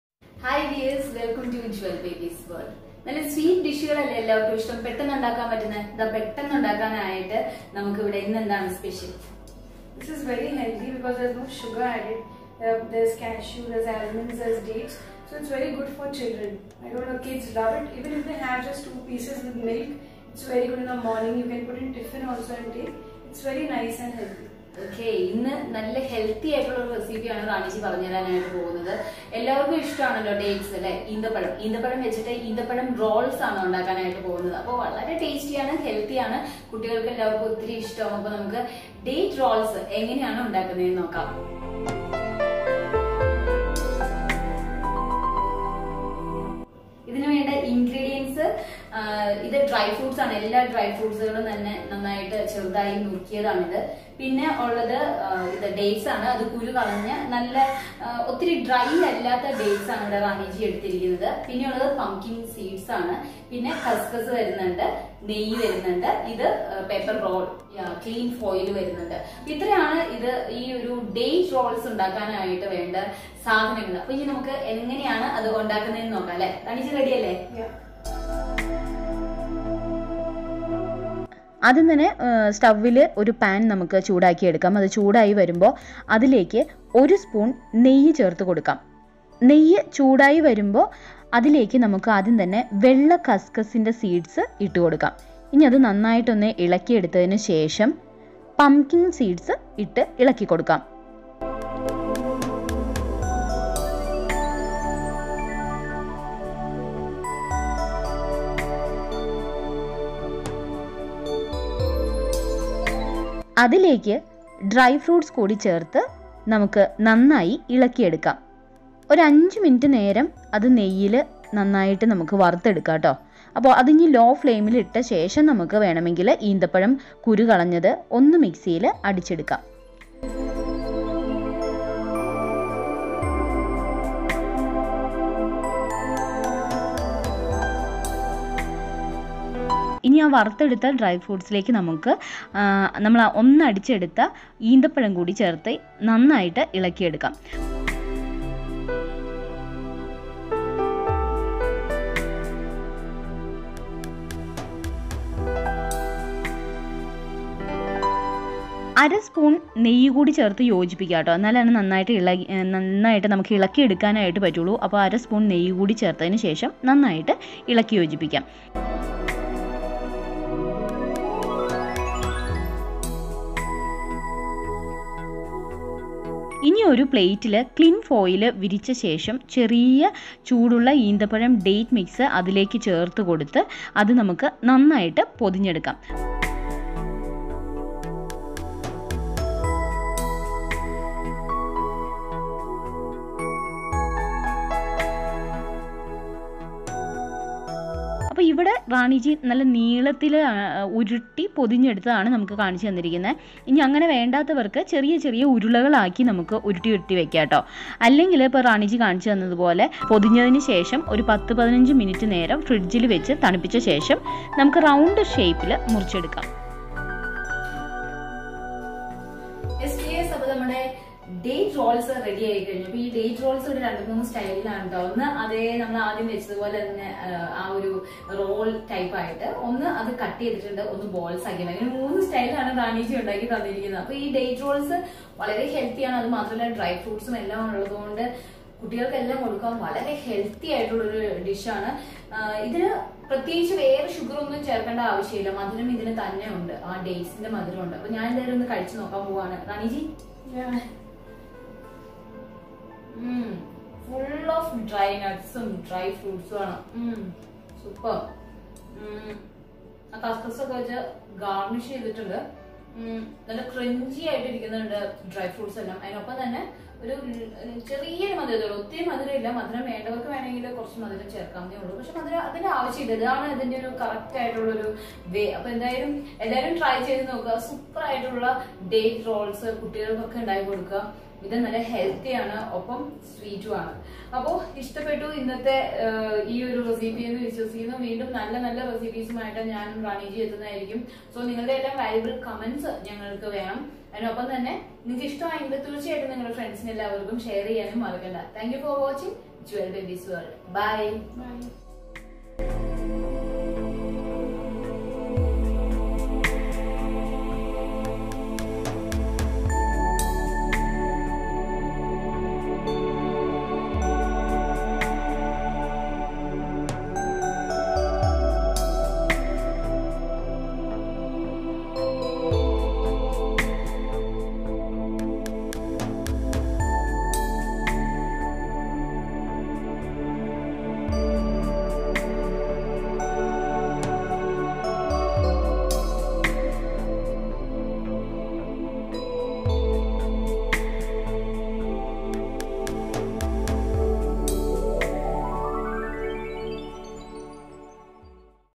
ഈന്തപ്പഴം റോൾ എളുപ്പത്തിൽ ഉണ്ടാക്കിയെടുക്കാം || How to prepare Date Rolls Easy At Home
ഈന്തപ്പഴം റോൾ ഇത്ര എളുപ്പമാണോ ? സ്വാദിഷ്ടവും ആരോഗ്യപ്രദവും || Date Rolls Easy At Home @jewelbabysworld